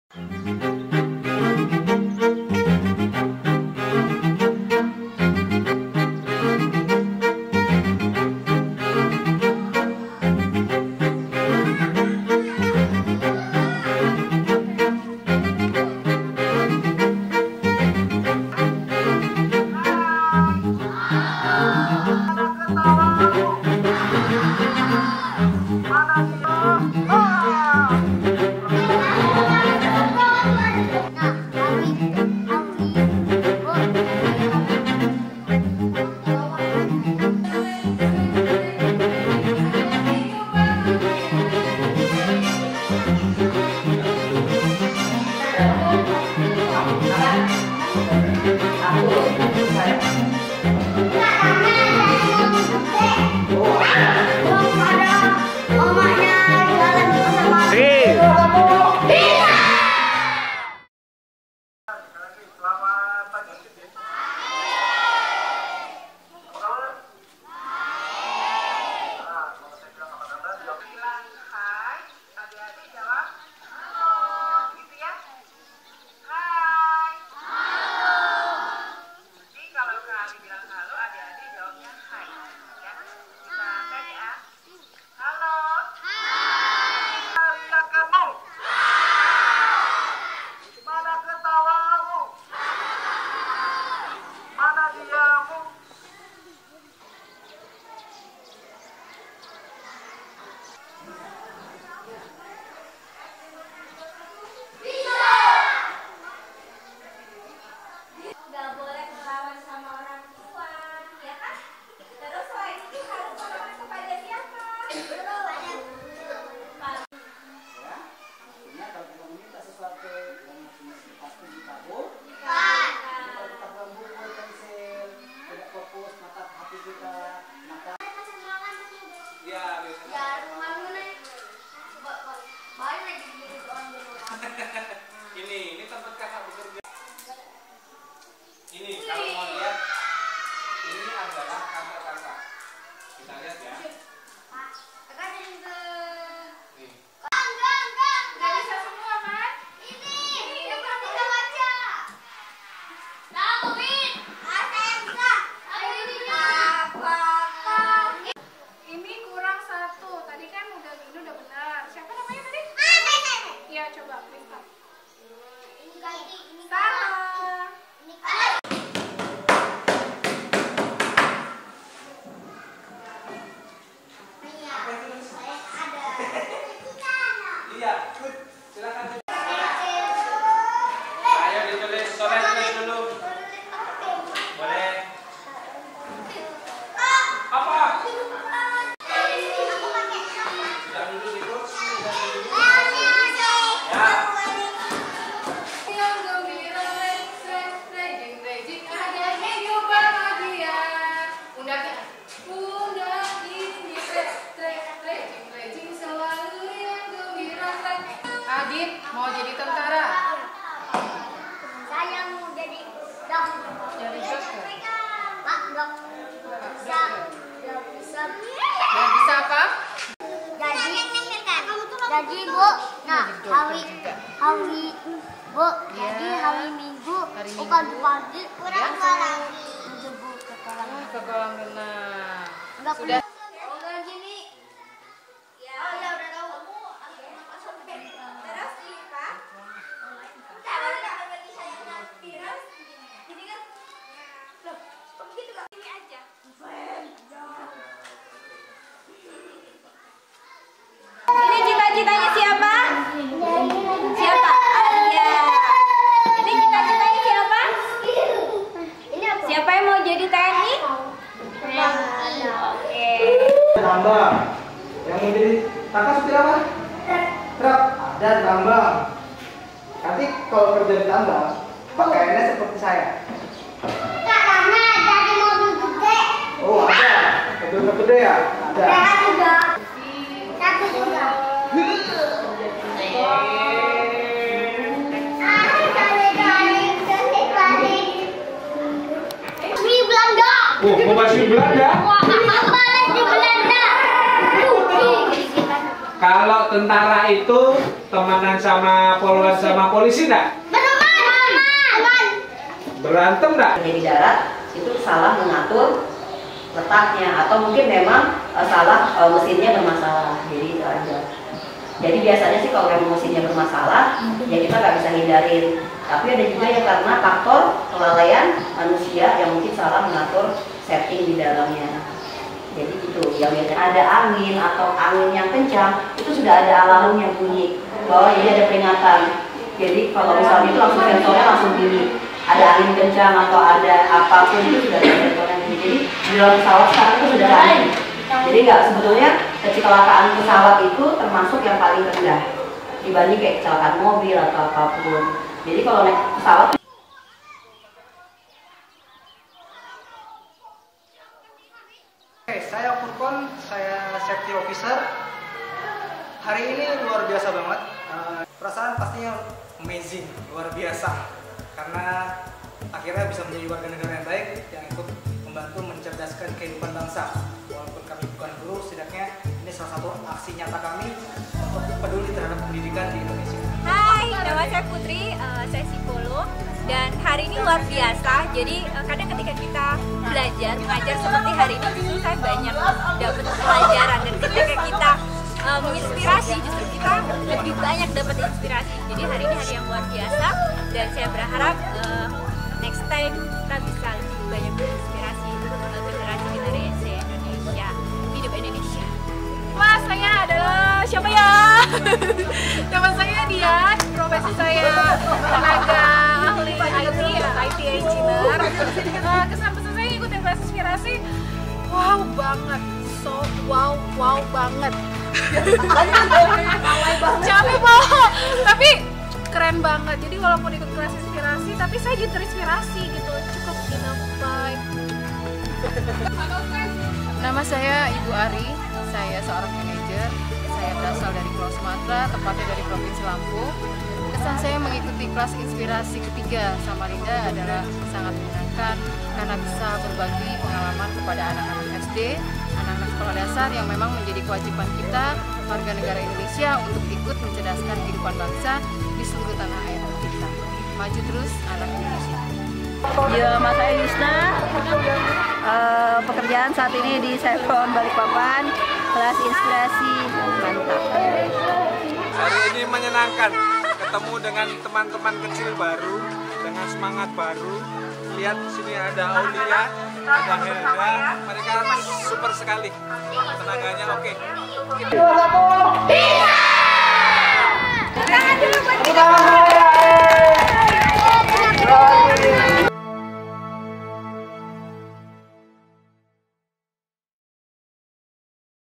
Kelas Inspirasi - SDN 021 Batu Besaung - Samarinda 2016
Mohon Maaf teman-teman Kelas Inspirasi Samarinda, video dokumentasi Kelas Inspirasi dari tim SDN 021 Batu Besaung baru selesai seadanya footage.
Semoga bisa lanjut~